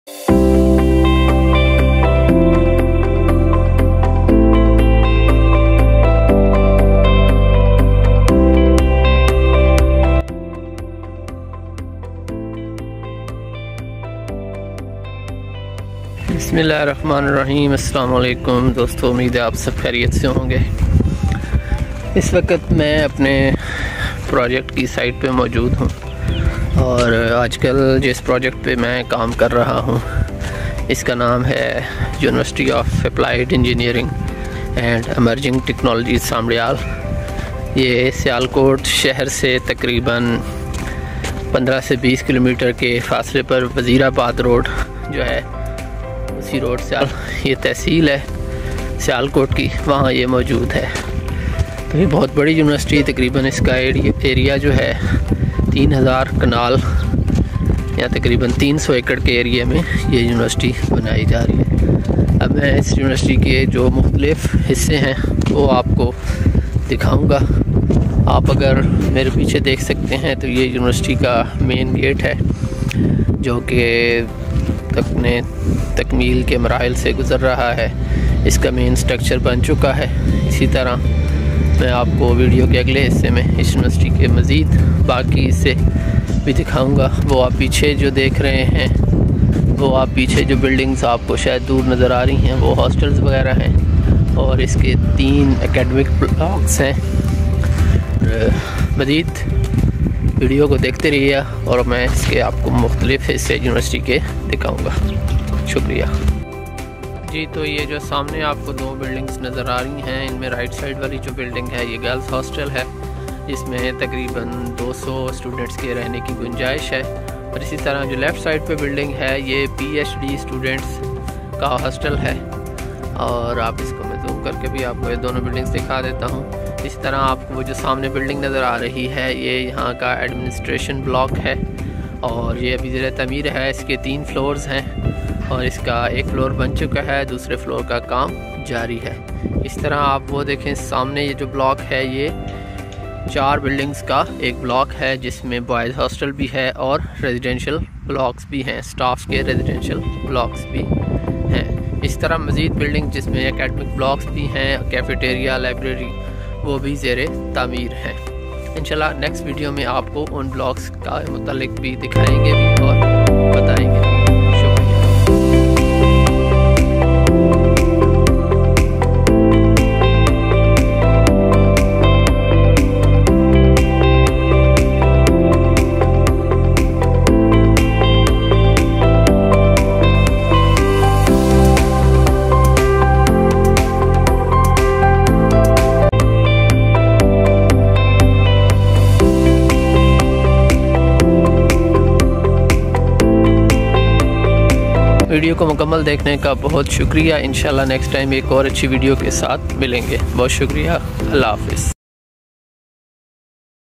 UAEET Sambrial
Infrastructure Development Authority of the Punjab (IDAP) will execute Establishment of the University of Applied Engineering and Emerging Technologies (UAEET), Sambrial to provide the best educational facilities to the individuals on a total area of 400 acres approximately. The university will have the following blocks:

Academic Block
Admin Block
Boys Hostel
PhD Students’ Housing
Small Family Housing
Medium Family Housing
International Faculty Housing
Girls Hostel
Library
Cafeteria
Prefabricated Building